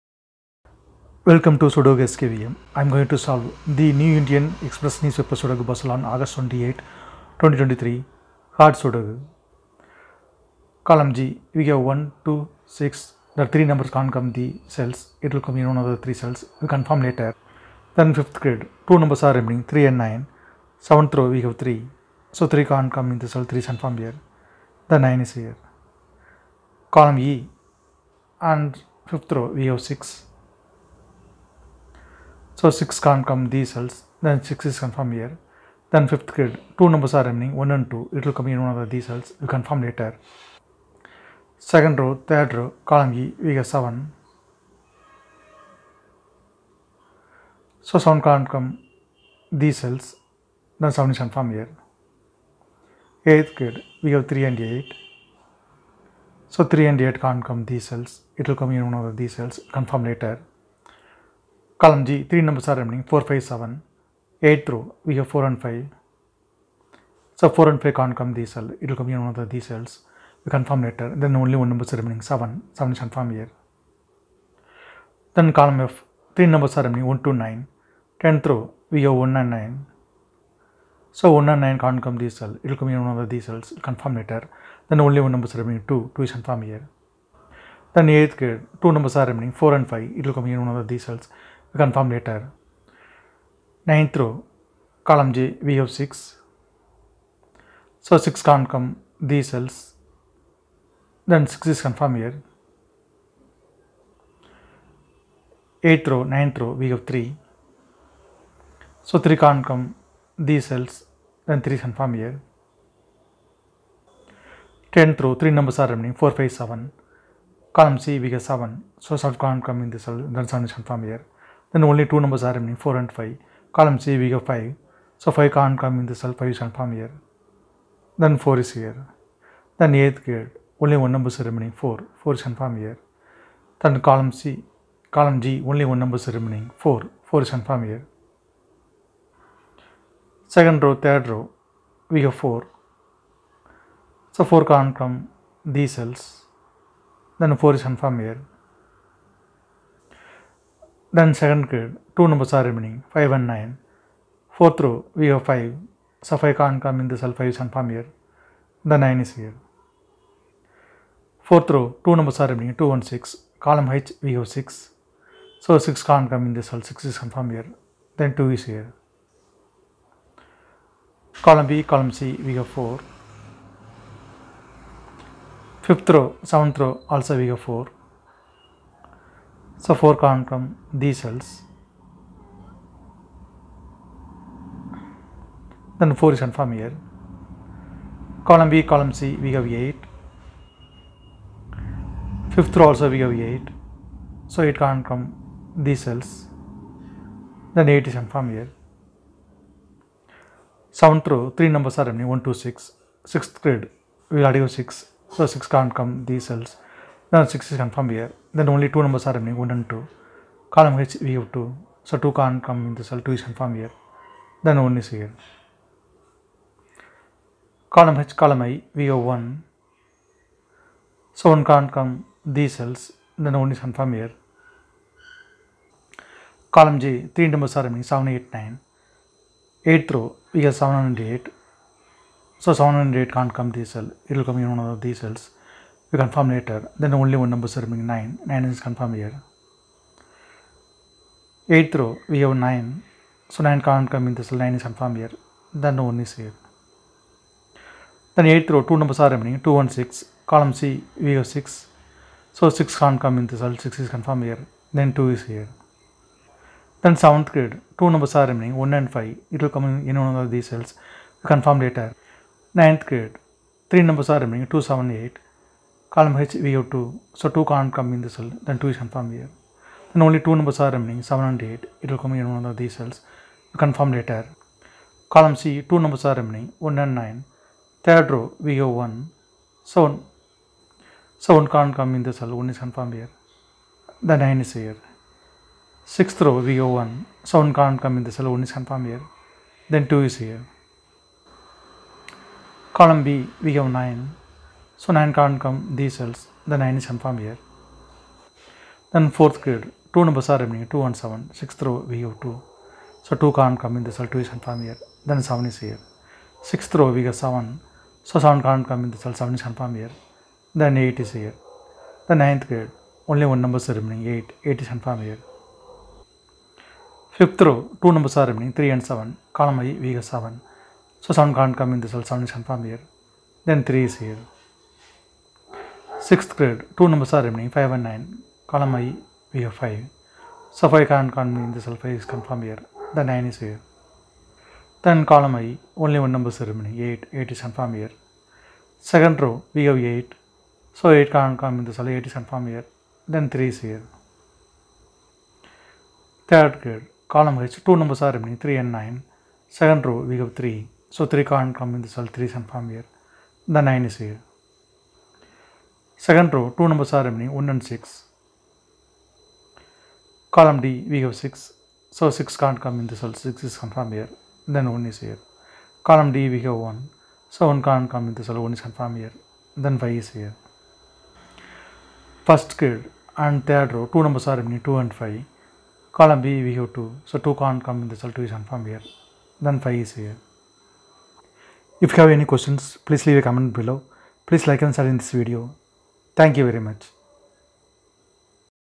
🔴 The New Indian Express Newspaper Hard Sudoku Answer, August 28, 2023 | Step-by-Step Solution
Today The New Indian Express Hard Sudoku Step-by-Step Solution for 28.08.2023